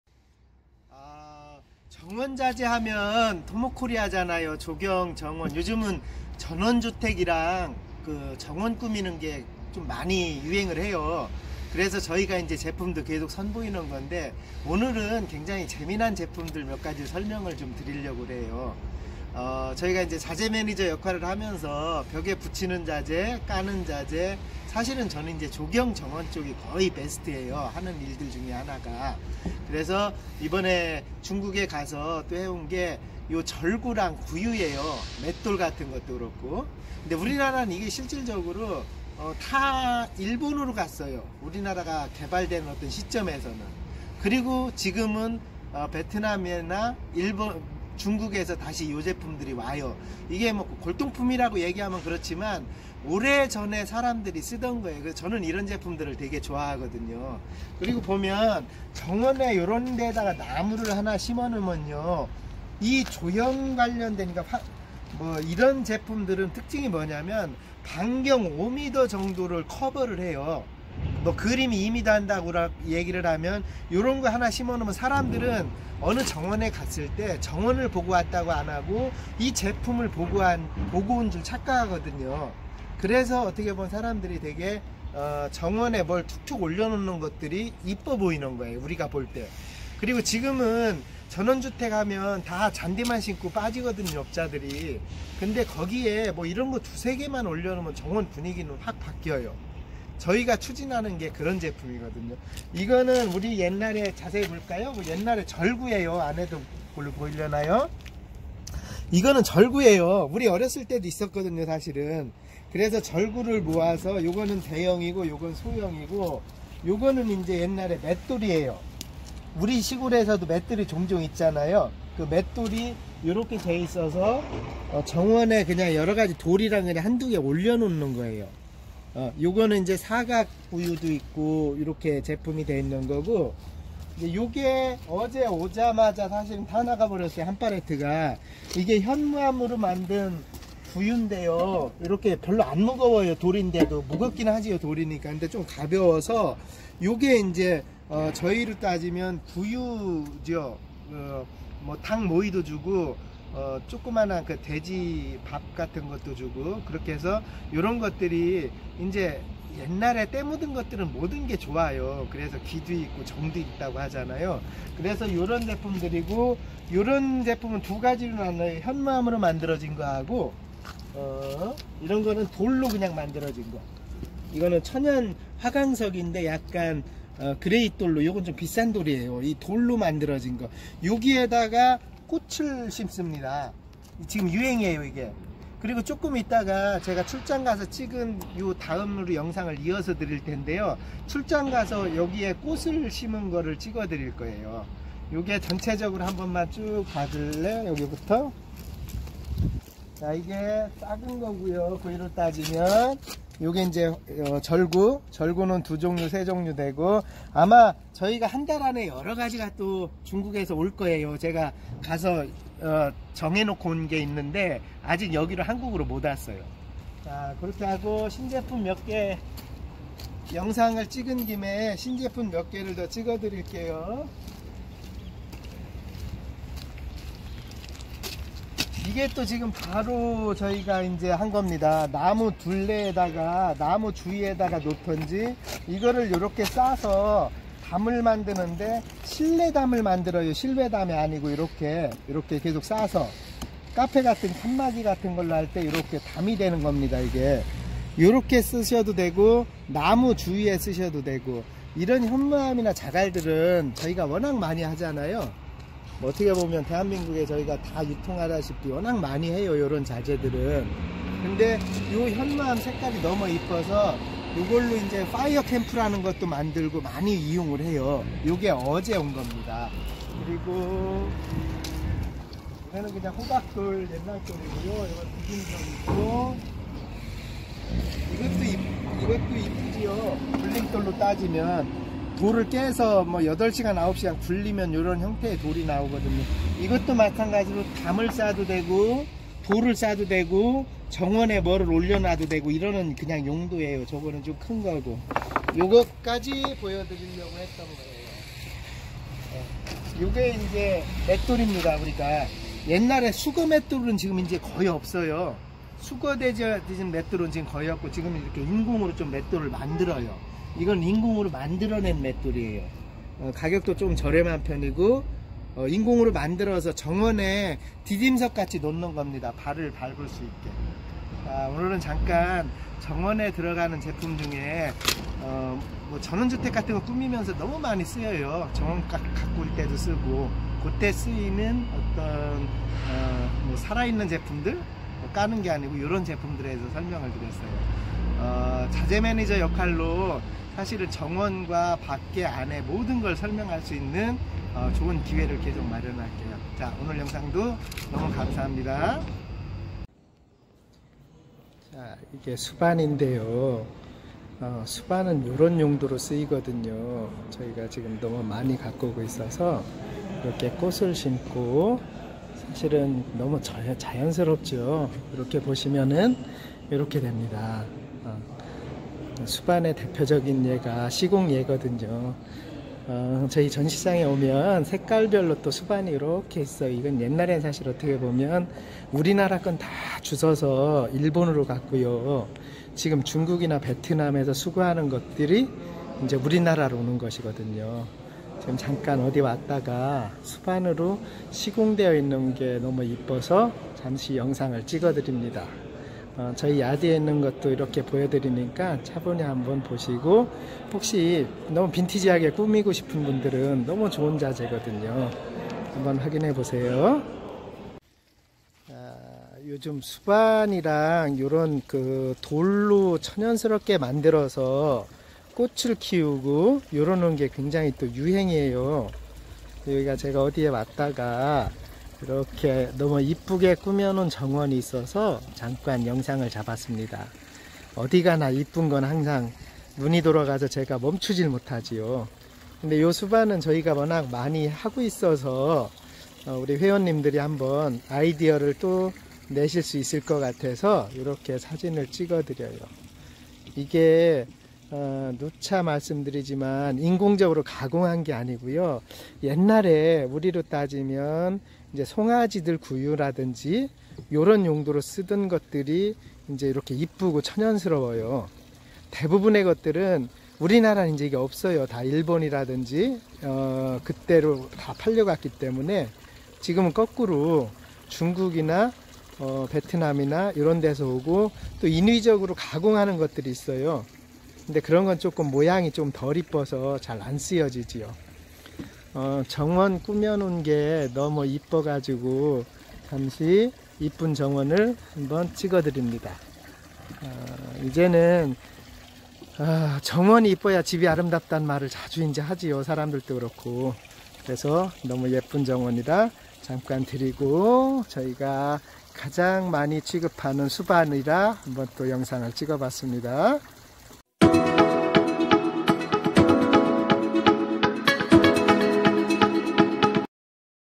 정원만들기 아름다운정원 전원주택 골동품 정원꾸미기 하나정도 있으면 품격이 달라지는 자재
오늘 포스팅은

맷돌
돌절구
구유 등입니다
정원 전원주택 골동품 정원꾸미기 하나정도 있으면 품격이 달라지는 자재입니다

전원주택이 유행 하면서 정원 문화가 바뀌고 있습니다.
그에 어울리는 자재를 연구하는게 또한 자재매니져의 몫인듯합니다.